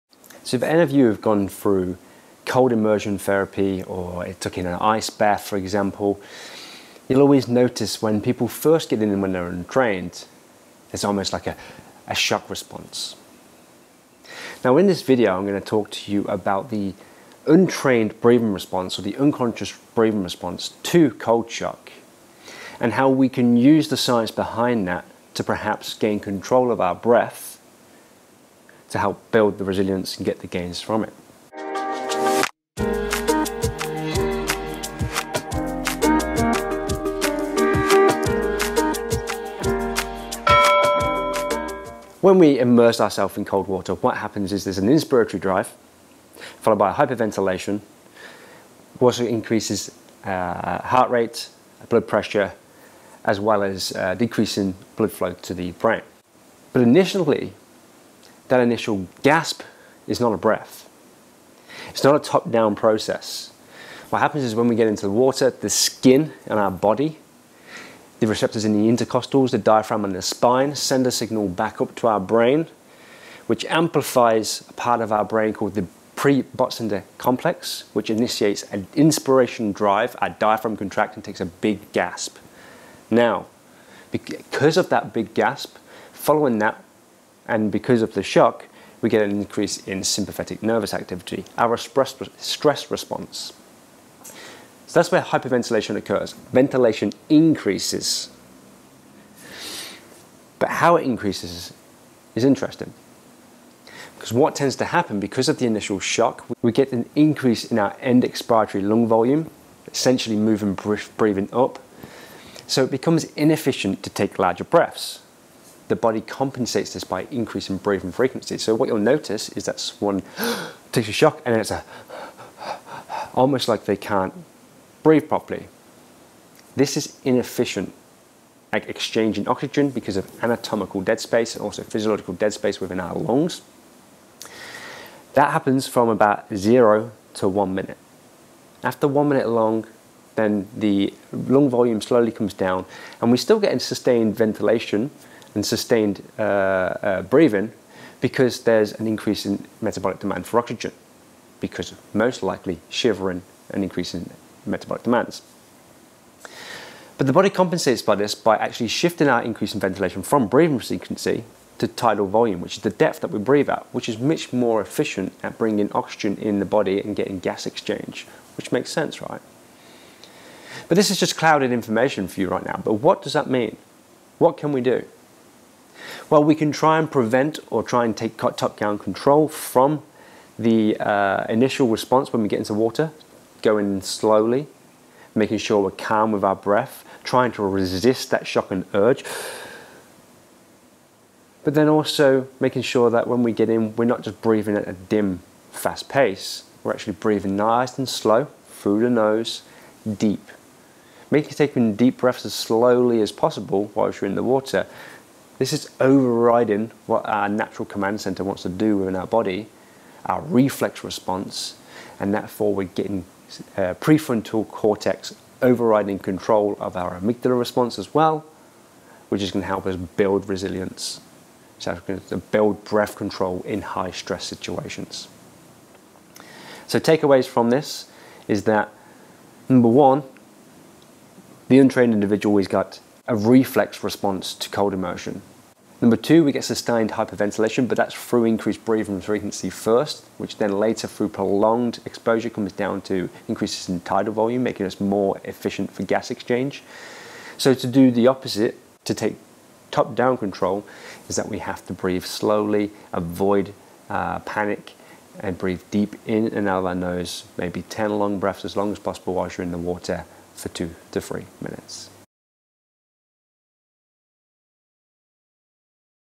How to Breathe During Cold Exposure | Tips for Overcoming the Breathing Response
Ice baths are all the rage at the moment, most likely due to Wim Hof's impact around the world.

However, the cold water plunge results in a vast physiological response to the brain and body. As a result, breathing becomes challenging. If not under control, it will only contribute to more stress, and you will struggle to reach the length required to get the result you wish.

Cold exposure over time can improve the immune system blood flow, reduce inflammation and help aid recovery from exercise; however, for many, it's all about the mind. That requires focus.

In this video, I will outline the breathing response. Hence, you know what to expect and then a simple technique to overcome the reflection. Please always make sure you perform cold exposure safely and do not if you have a history of cardiac disease. Consult your physician first.